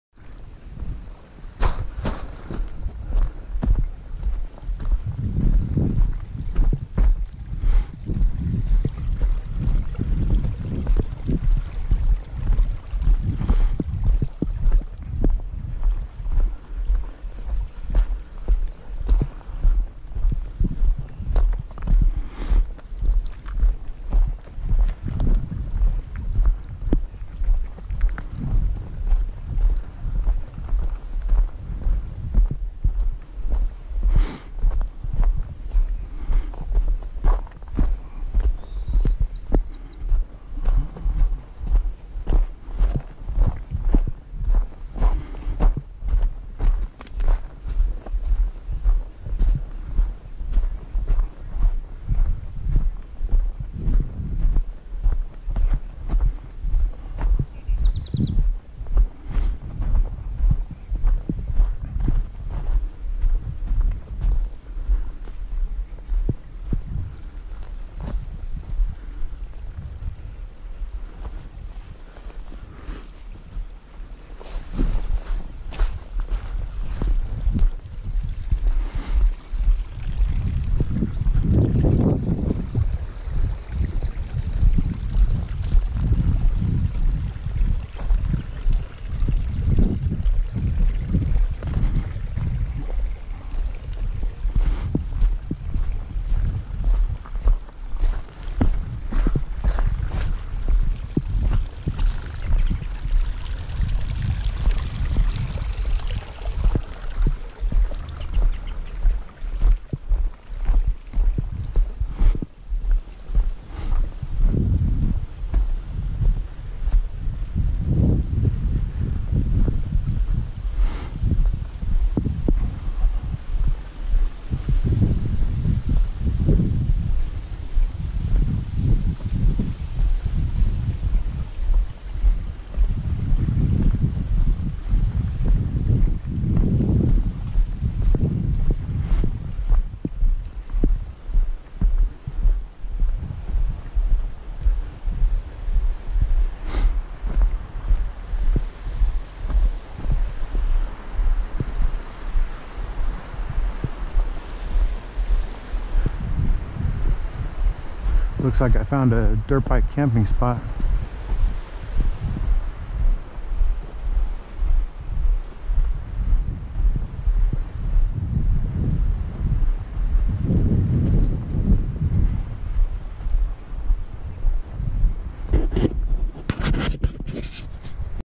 Hiking around the carson river part 2
Hiking the carson river and found a cool camping spot!